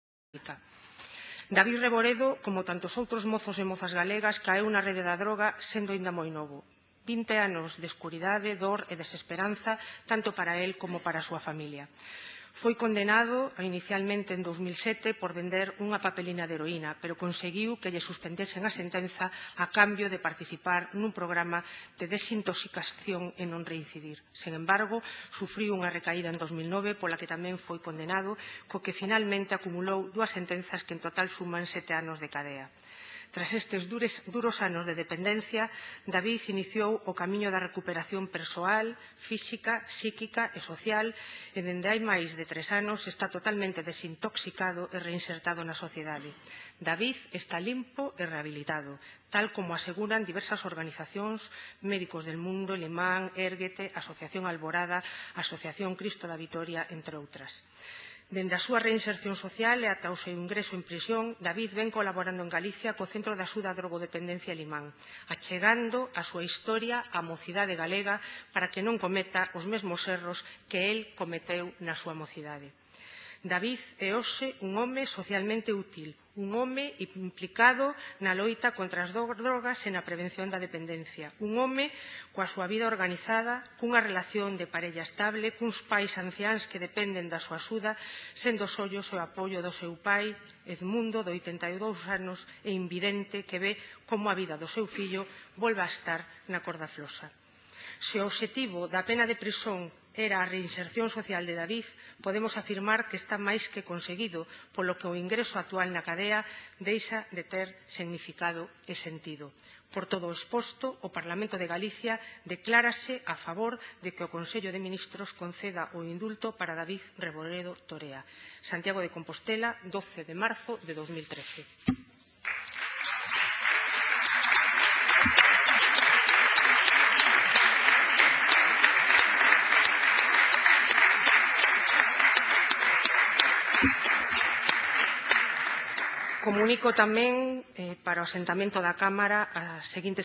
Indulto para David Reboredo. Declaración institucional do Parlamento de Galicia
En apoio ao indulto a Davide Reboredo.Declaración institucional do Parlamento de Galicia